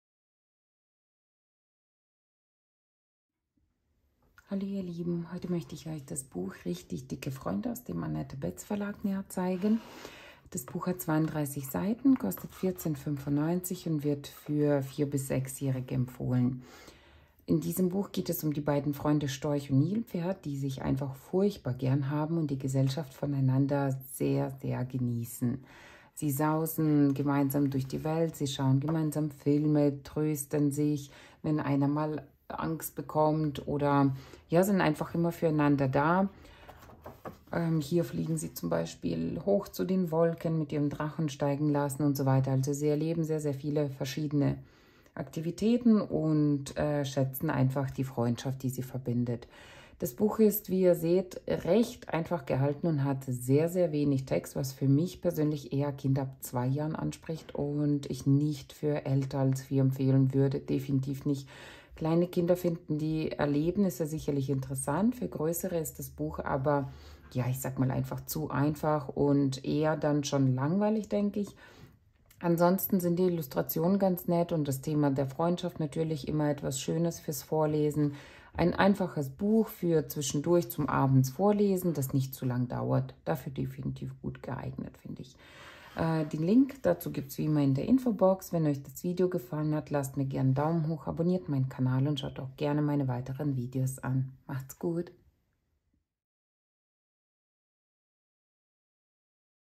Richtig dicke Freunde Kinderbuch/ Bilderbuch/ Buch/ Geschichte/ Freundschaft/Buchvorstellung/ lesen
Richtig dicke Freunde
Verlag: annette betz
Seitenanzahl: 32 Seiten
Preis:  14,95 €
Altersempfehlung: ab 4-6 Jahre

Solltet ihr über diesen Link bestellen, erhalte ich eine kleine Provision.
Ihr zahlt nichts drauf, der Preis bleibt gleich.